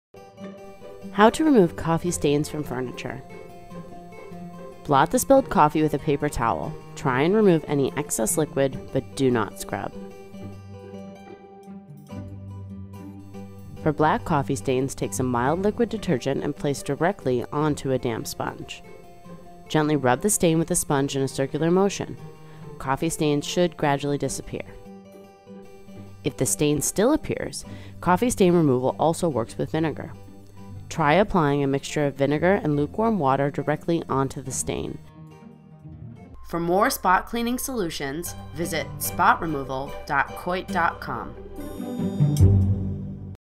How to Clean Upholstery from coffee spot
save yor time and money, if you make a spot on your upholstery sofa or chair , you still can save it by yourself. follow this video how to do it